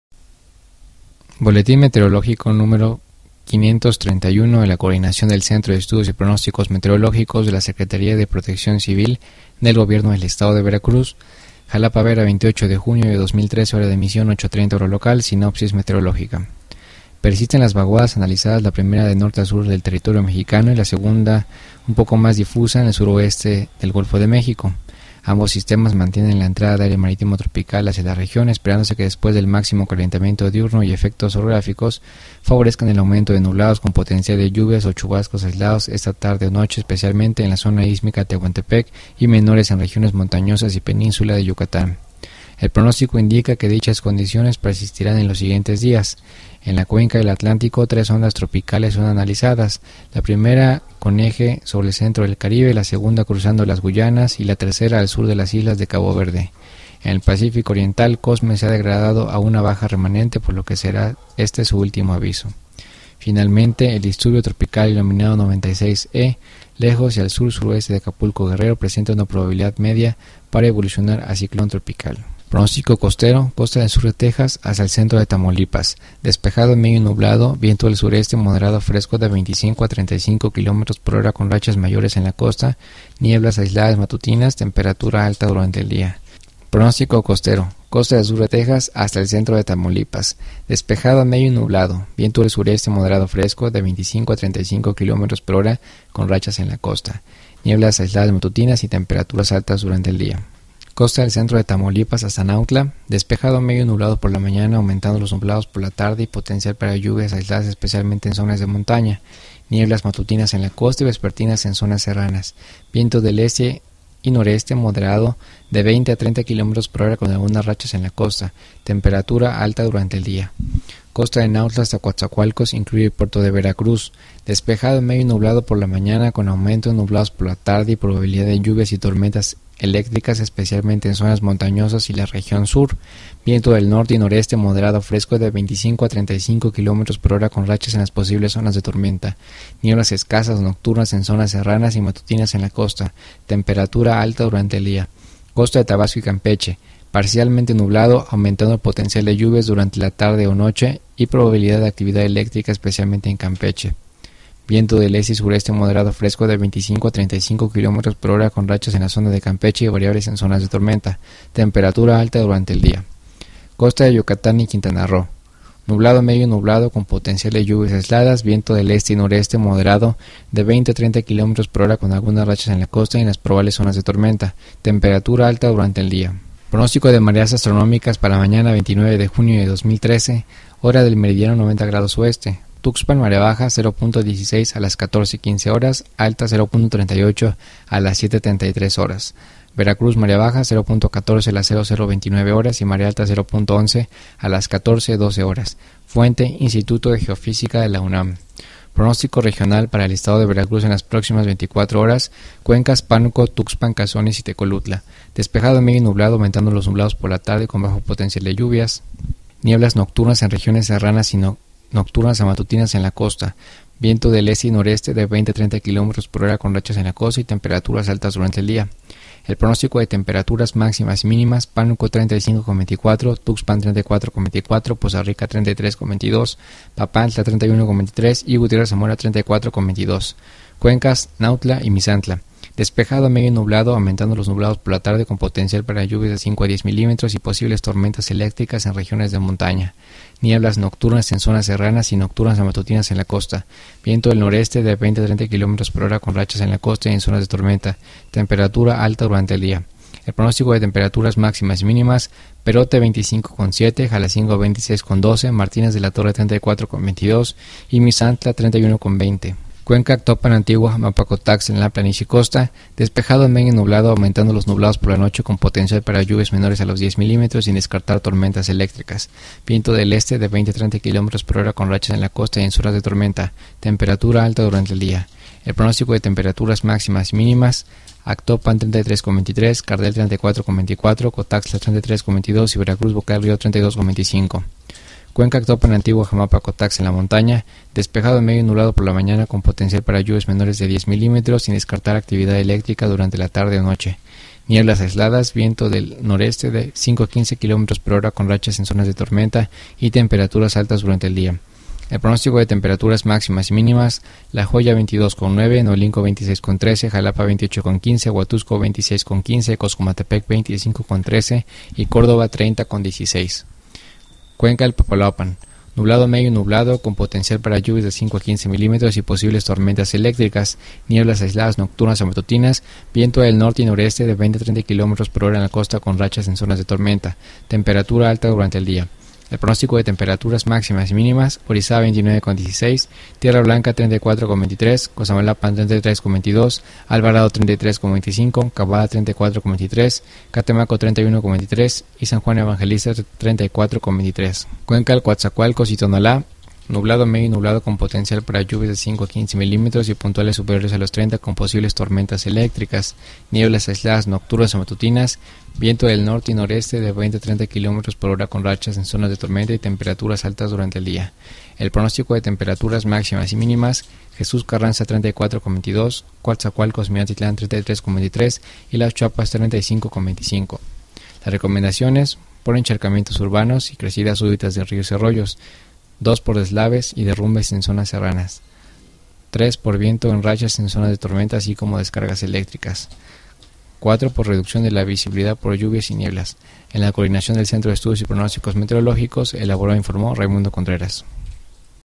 Estado del tiempo 28 de junio de 2013
Persisten las vaguadas analizadas, la primera de norte a sur del territorio mexicano y la segunda ha esta hora un poco mas difusa, en el suroeste del Golfo de México. Ambos sistemas  mantienen la entrada de aire marítimo tropical hacia la región, esperándose que despues del máximo calentamiento diurno y apoyado por los efectos orográficos favorezcan el aumento de los nublados con potencial de lluvias o chubascos aislados esta tarde-noche especialmente en la zona ístmica de Tehuantepec y menores en regiones montañosas y Pensínsula de Yucatán. El pronóstico indica que dichas condiciones persistirán en los siguientes días. En la cuenca del Atlántico, tres ondas tropicales son analizadas, la primera con eje sobre el centro del Caribe, la segunda cruzando las Guyanas y la tercera al sur de las islas de Cabo Verde. En el Pacífico Oriental, "COSME" se ha degradado a una baja remanente por lo que será su último aviso. Finalmente, el disturbio tropical denominado 96-E lejos y al sur-suroeste de Acapulco, Gro. presenta una probabilidad media para evolucionar a ciclón tropical.